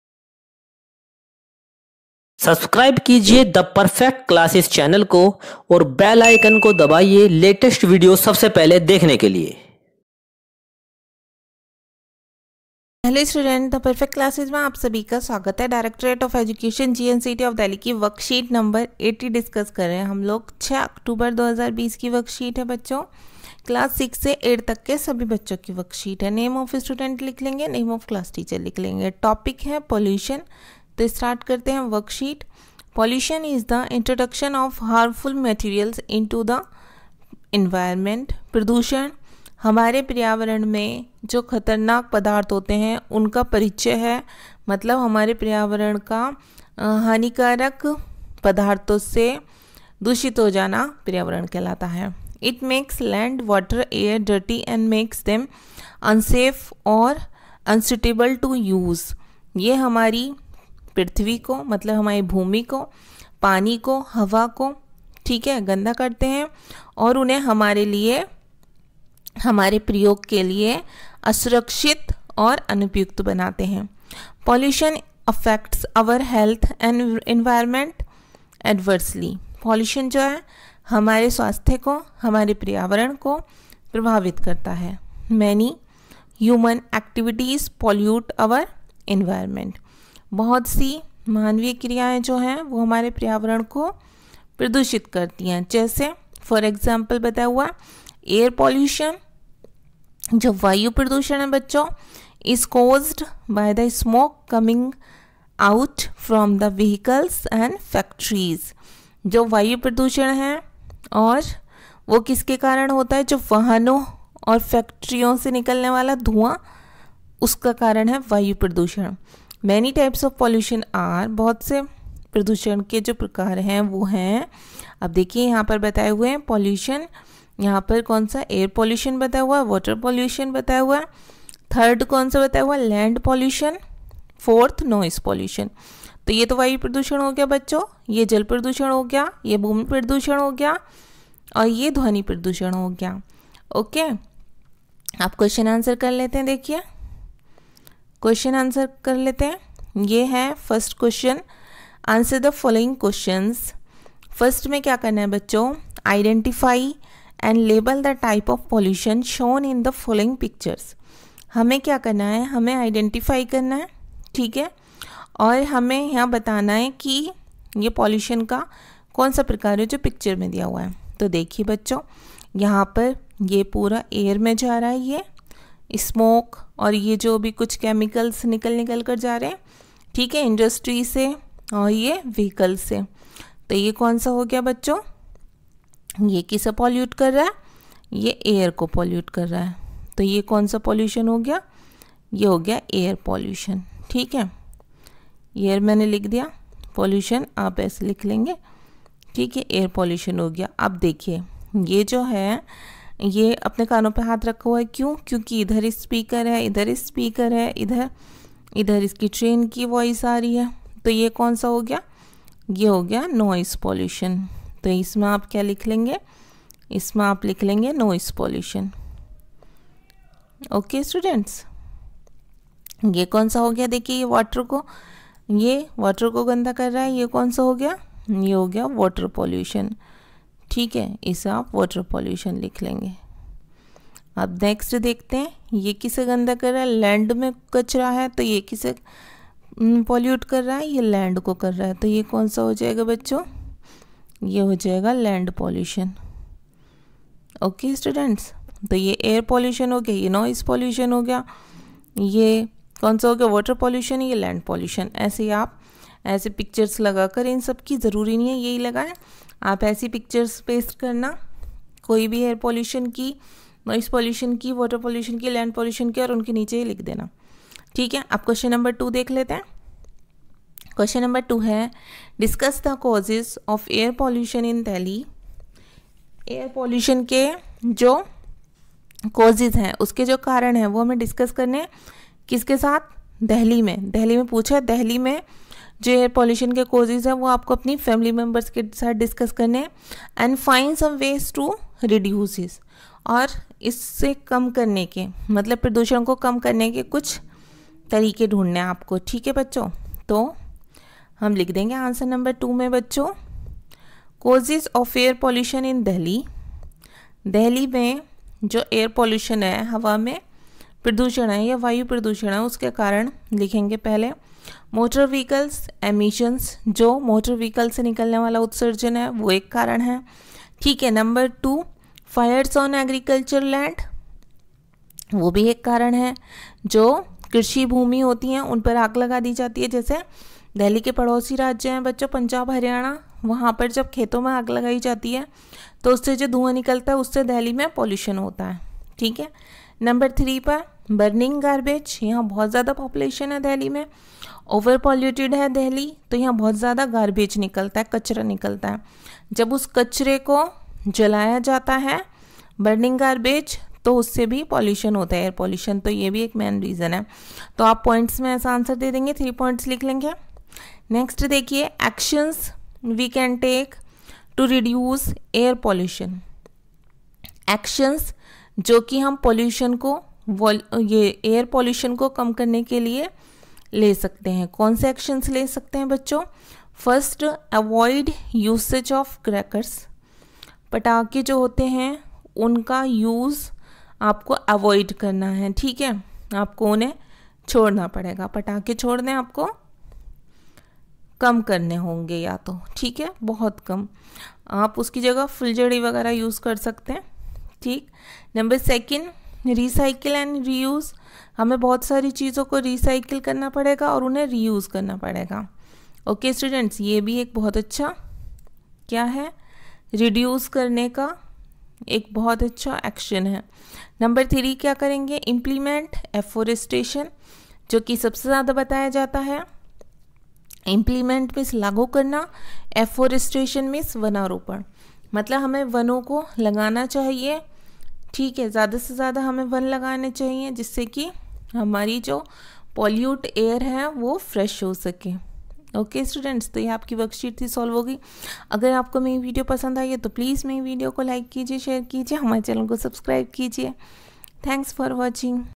DOE DELHI WORKSHEET 80 CLASS 6 TO 8 (ENGLISH MEDIUM)TUESDAY 6 OCTOBER 2020
maine is video ke madhiyam se DOE, DELHI  WORKSHEET 80 FOR CLASS 6  TO 8 OCTOBER  2020 KO SAMJHAYA HAI..